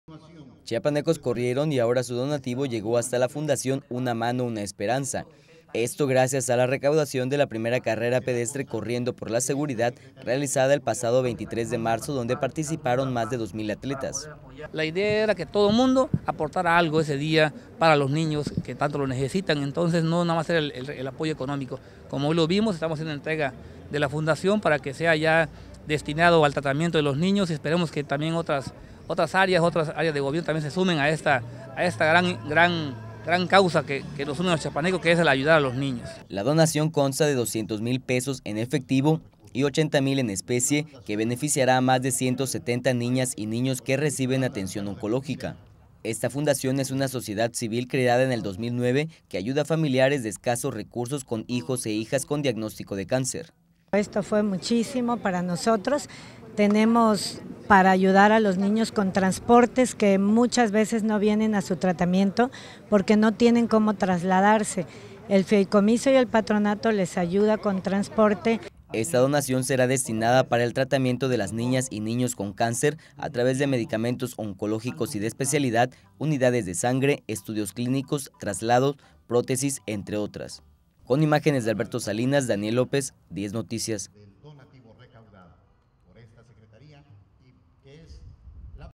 La SSPC Chiapas entrega donativo a la fundación "Una mano, una esperanza"
La fundación "Una mano, una esperanza" que ayuda a niños con cáncer fue beneficiada gracias a la recaudación de la primera carrera pedestre "Corriendo por la seguridad" organizada por la Secretaría de Seguridad y Protección Ciudadana el pasado 23 de marzo dónde participaron más de dos mil atletas.
La donación fue de 200 mil pesos en efectivo y 80 mil en especie, que beneficiará a más de 170 niñas y niños que reciben atención oncológica.